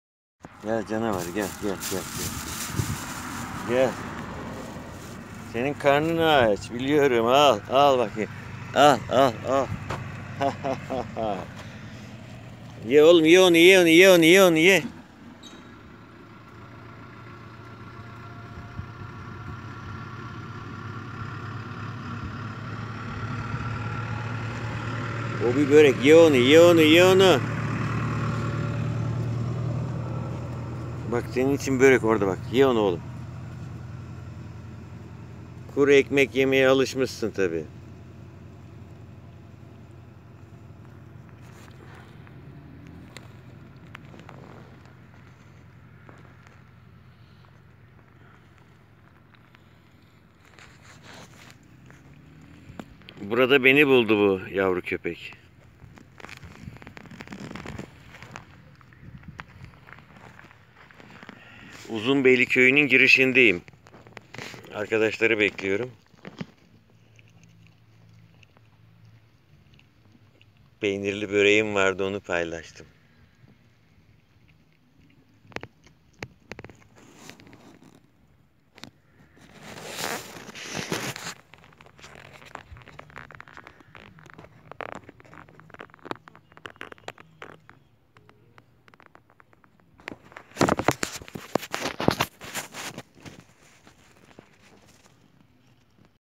Minik Köpeğle ekmeği paylaşmak (21.08.2017)
Gazi Tepesi- Şuhur arasındaki yürüyüşcüleri Polatlı'ya 74 Km uzaklıktaki Uzunbeyli Köyü yakınında beklerken bu yavru çıktı geldi yanıma. Zayıftı. Onunla kısıtlı olan yiyeceklerimi paylaştım. Uzaktan beni görüp koşarak gelmesi şaşırtıcıydı.Hayvanlarla da olsa paylaşmak güzeldir.